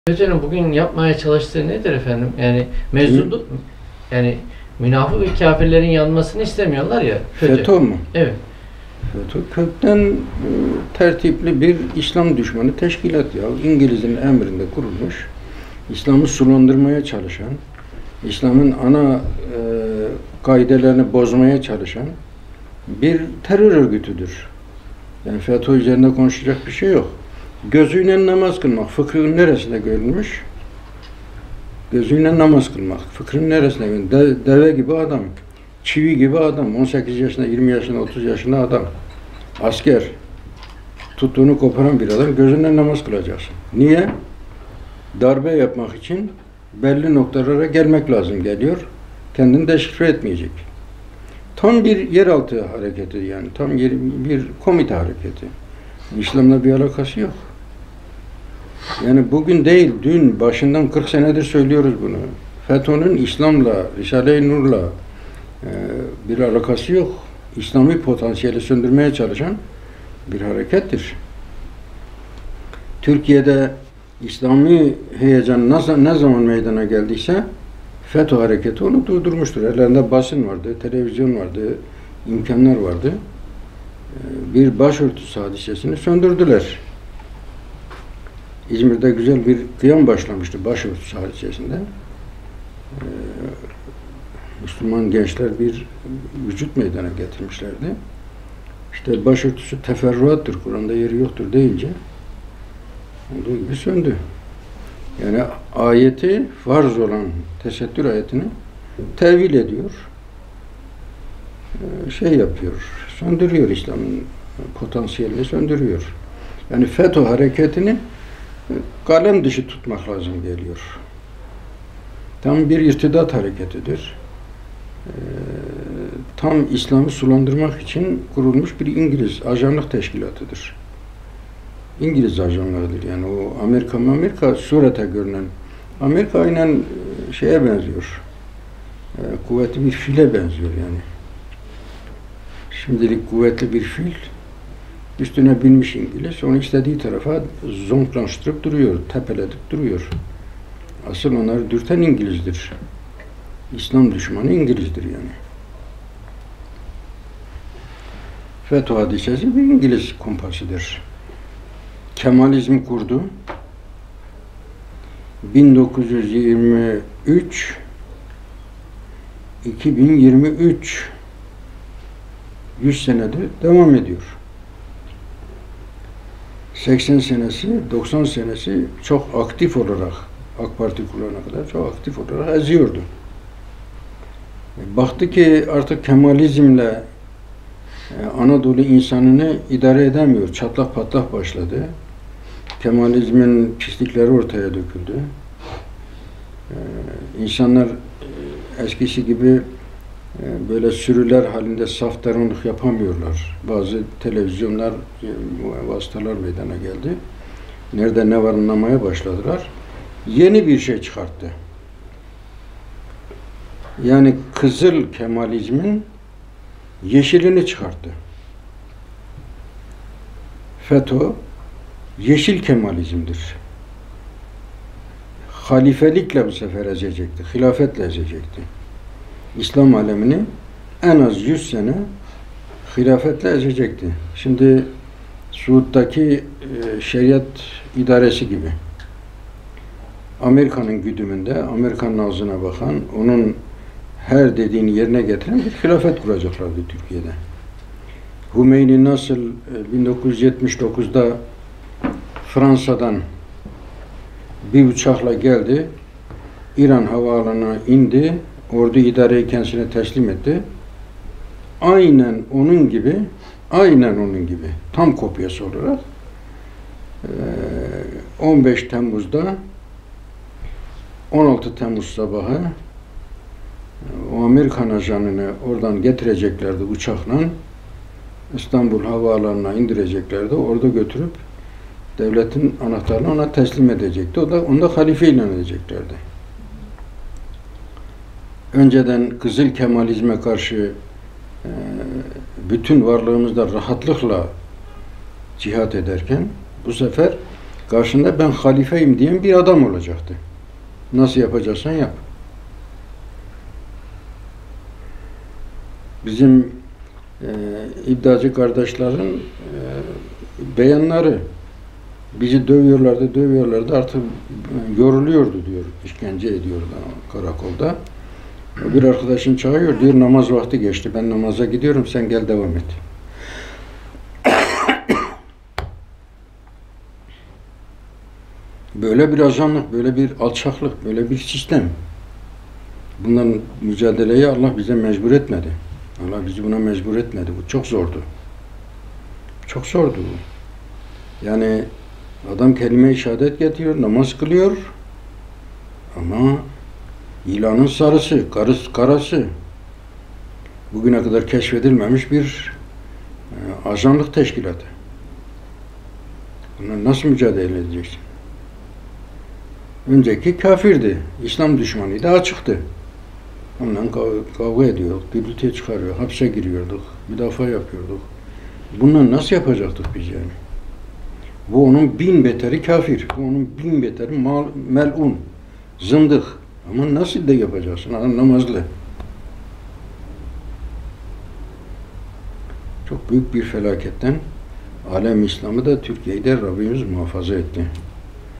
[812] Fetö ingiliz tarafından kurulan terör örgütüdür (Müslim Gündüz Efendi)
Şayet 15 Temmuz muvaffak olsaydı 16 Temmuz sabahı fetullah gülen halife olarak İstanbul'a getirilecekti.